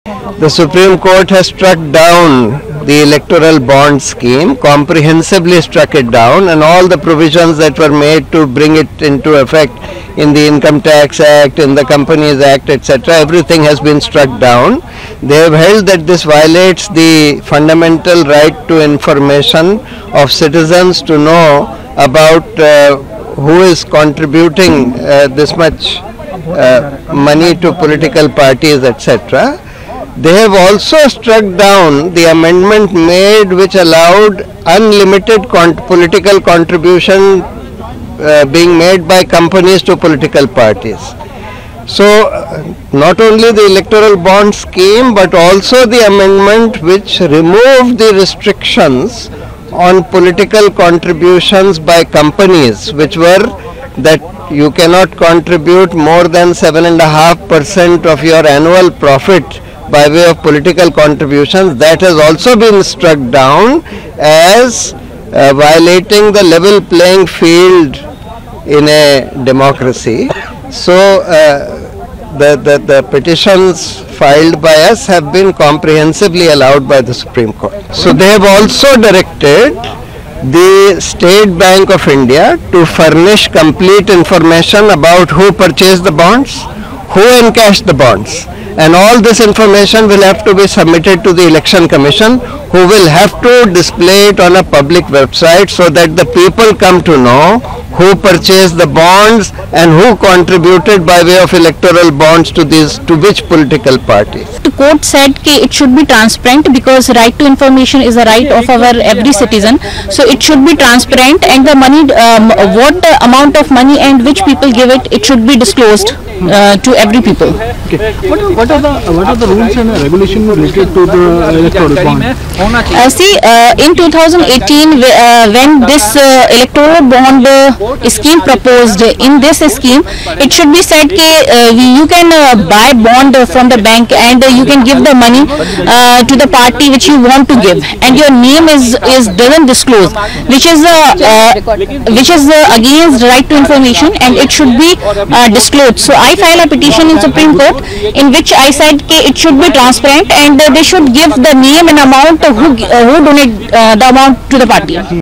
“Unconstitutional…” चुनावी बांड योजना पर Supreme court ने सुनाया बड़ा फैसला
“Unconstitutional…” Supreme Court gives big verdict on Electoral Bonds Scheme |Hit TV Today